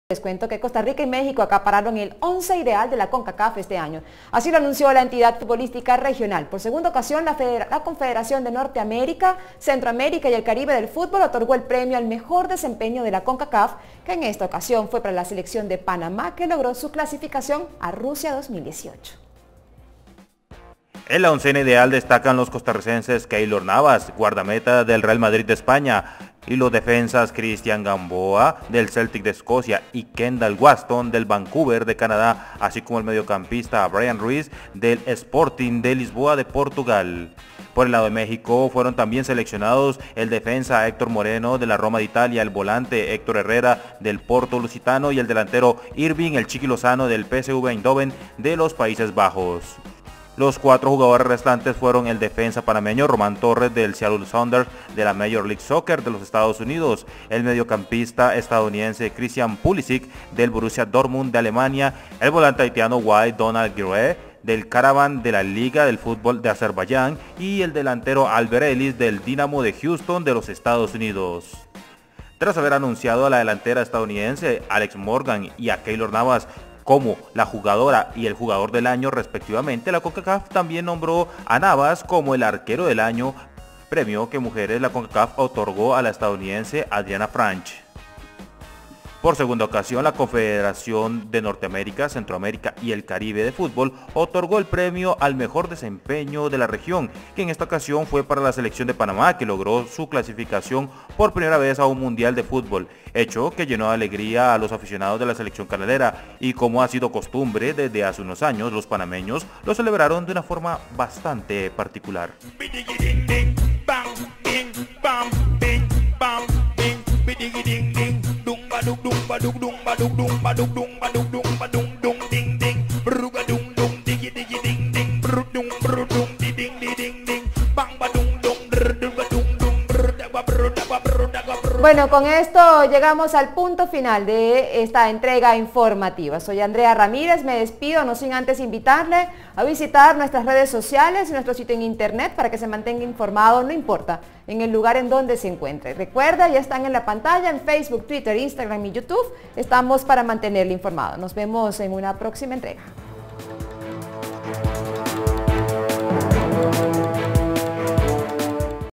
Costa Rica y México acapararon el “once ideal” de la Concacaf
Costa Rica y México acapararon el “once ideal” de la Concacaf este año, así lo anunció la entidad futbolística regional.

Por segunda ocasión la Confederación de Norteamérica, Centroamérica y el Caribe de Fútbol otorgó el Premio al Mejor Desempeño de la Concacaf, que en esta ocasión fue para la selección de Panamá que logró su clasificación, por primera vez, a un Mundial mayor de fútbol.

En la ocena ideal destacan los costarricenses Keylor Navas, Guardameta de Real Madrid de España, los defensas, Cristian Gamboa del Celtic de Escocia y  Kendall Watson del Vancouver de Canadá y el mediocampista Bryan Ruiz del   Sporting Lisboa  de Portugal.

Por México fueron seleccionados, el defensa Héctor Moreno de la Roma de Italia, el volante Héctor Herrera, Porto lusitano y el delantero Hirving Lozano de PSV Eindhoven de Países Bajos.

Los cuatro jugadores restantes fueron el defensa panameño Román Torres del Seattle Sounders de la Mayor League Soccer de los Estados Unidos, el mediocampista estadounidense Christian Pulisic del Borussia Dortmund de Alemania, el volante haitiano Wilde-Donald Guerrier Qaraban de la liga de futbol de Azerbaiyán y el delantero hondureño Alberth Elis de Dynamo de Houston de los Estados Unidos.

Tras haber anunciado a la delantera estadounidense Alex Morgan y Keylor Navas como la jugadora y el jugador del año respectivamente, la Concacaf también nombró Navas como el arquerodel año, premio que en mujeres la Concacaf otorgó a la estadounidense Adrianna Franch.

Por segunda ocasión la Confederación de Norteamérica, Centroamérica y el Caribe de Fútbol otorgó el Premio al Mejor Desempeño de la Concacaf, que en esta ocasión fue para la selección de Panamá que logró su clasificación, por primera vez, a un Mundial mayor de fútbol, hecho que llenó de alegría a los aficionados de la selección canalera y como ha sido costumbre desde hace unos años, los panameños celebran de una forma algo particular

Redacción CB24